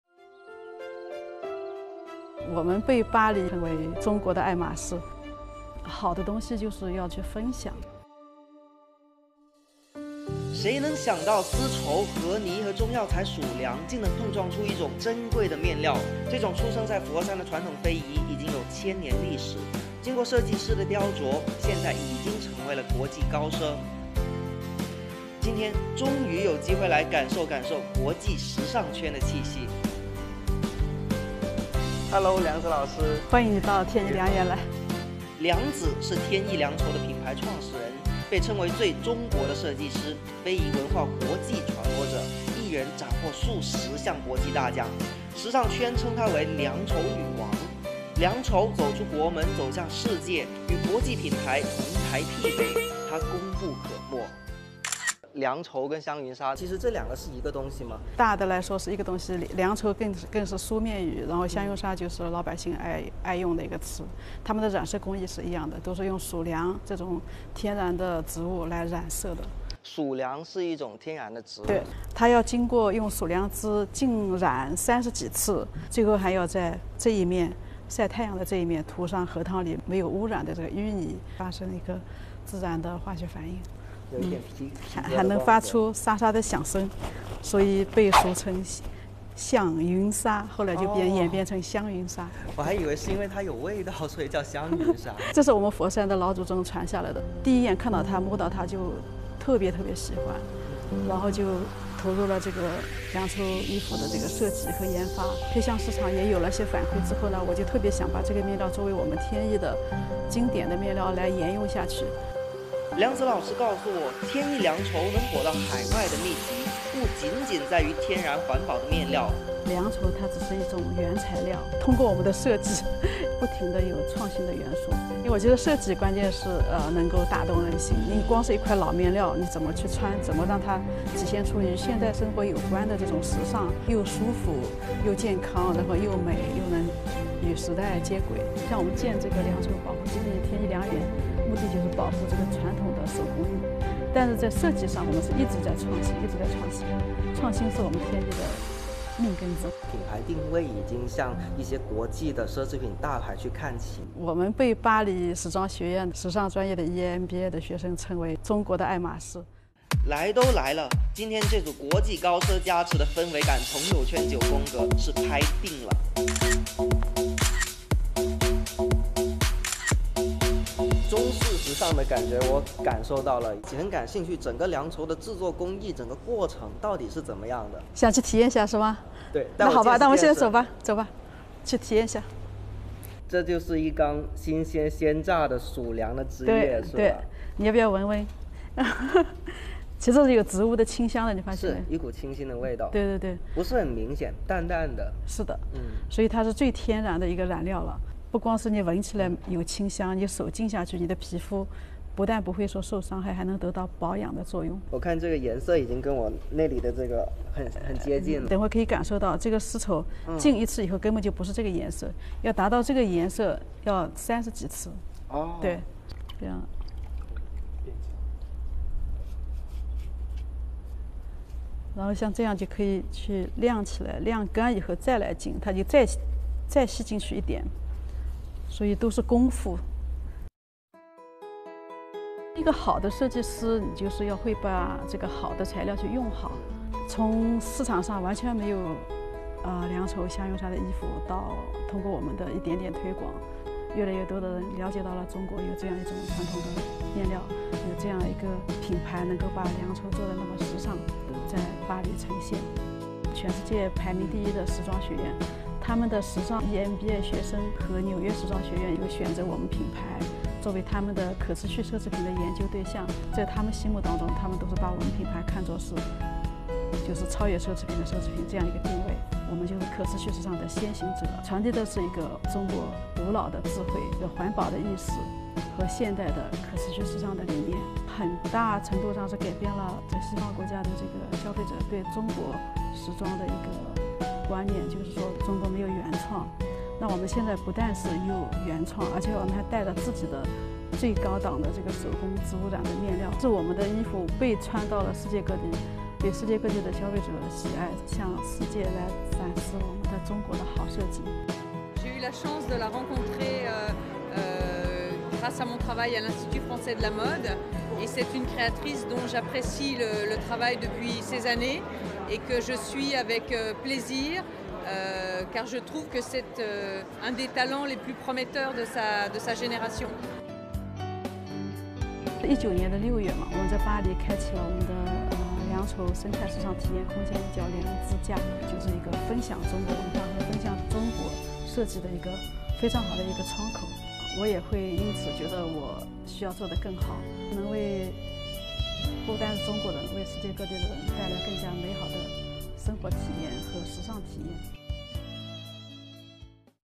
Guangdong’s Gamblered Silk in Paris
Designer Liang Ziyu met Gamblered Silk in 1995 for the very first time and fell in love with it instantly. In 2003, as Liang was invited to attend the Paris Fashion Week in France, she brought the Gamblered Silk to the world’s fashion stage.